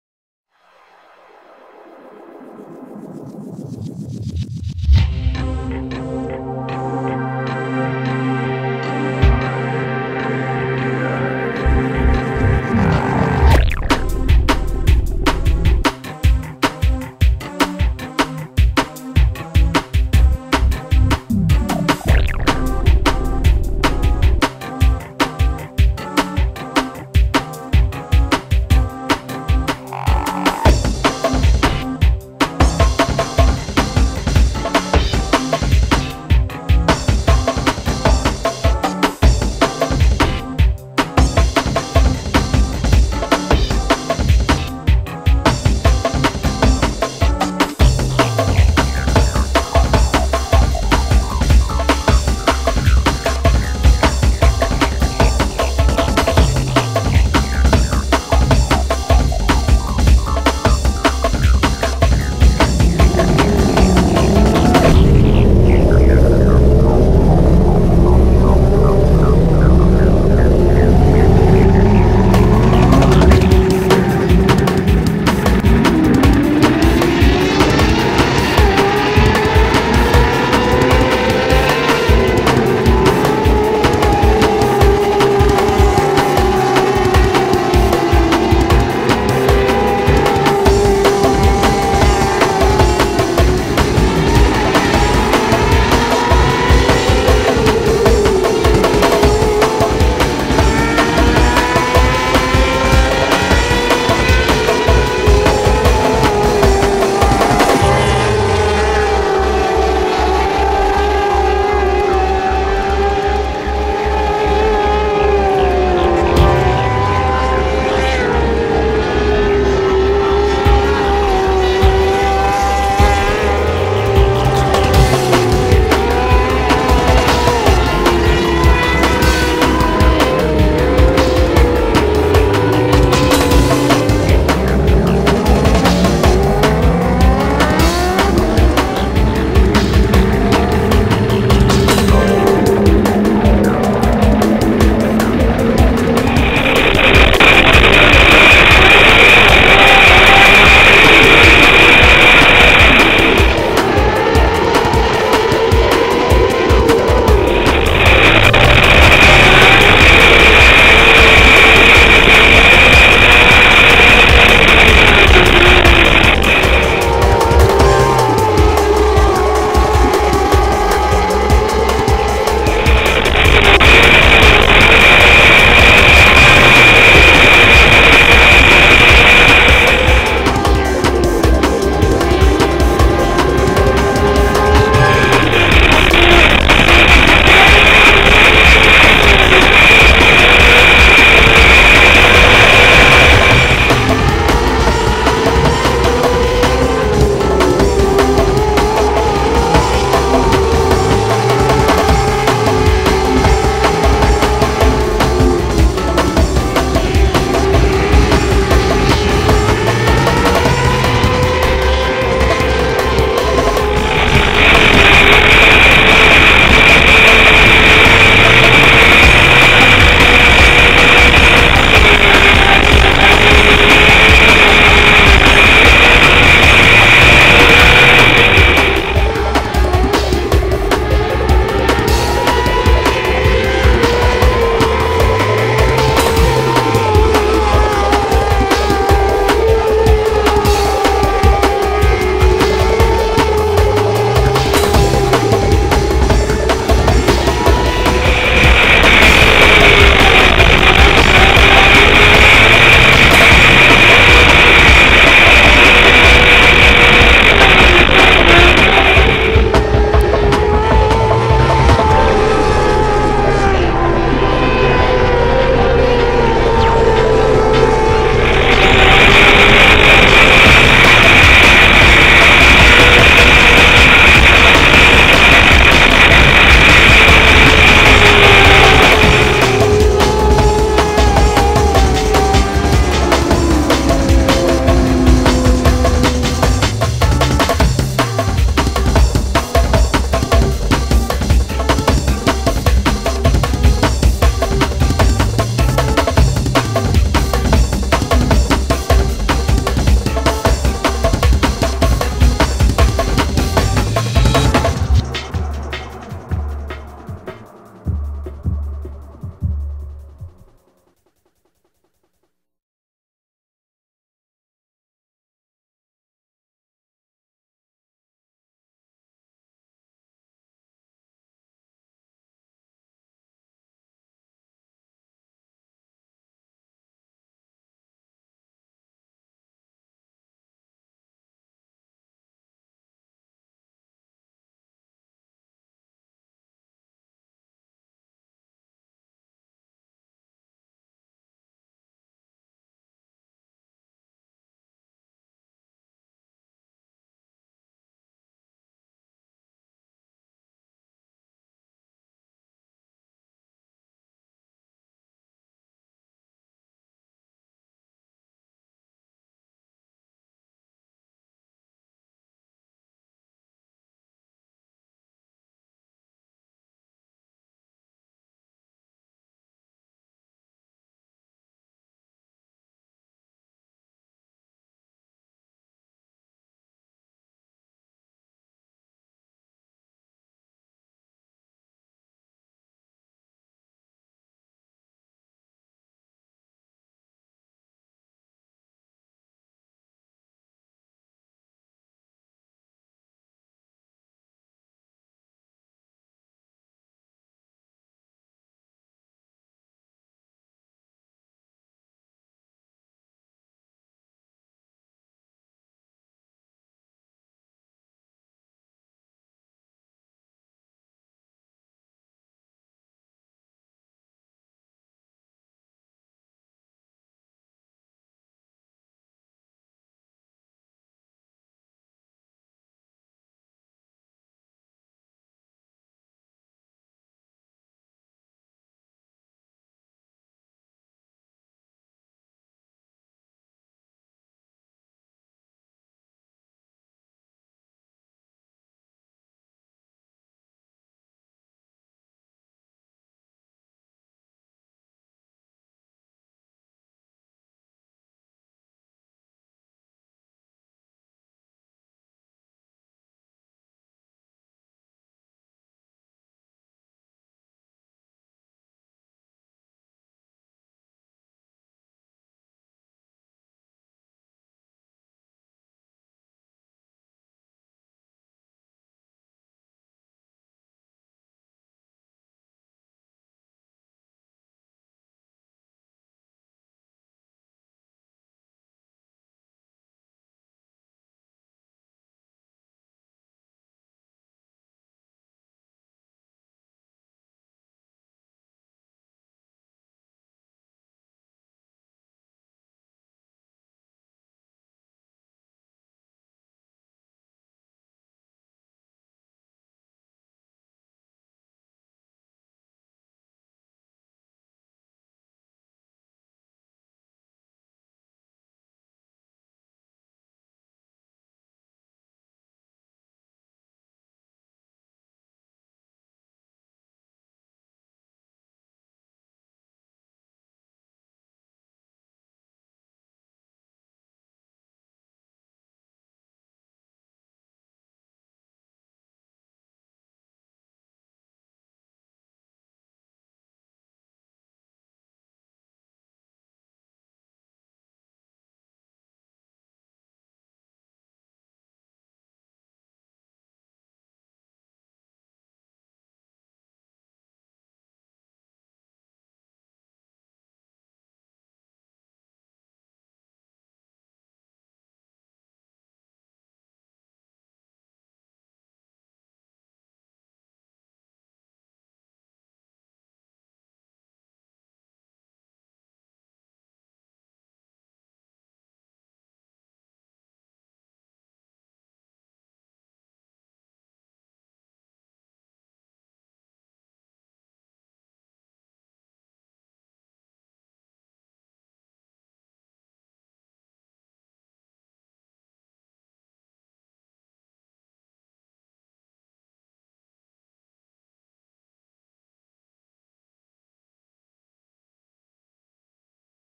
paradise speedway may 15, 2010 part one_0001.wmv
paradise speedway may 15, 2010 highlights from hot laps and heats 600 micro sprints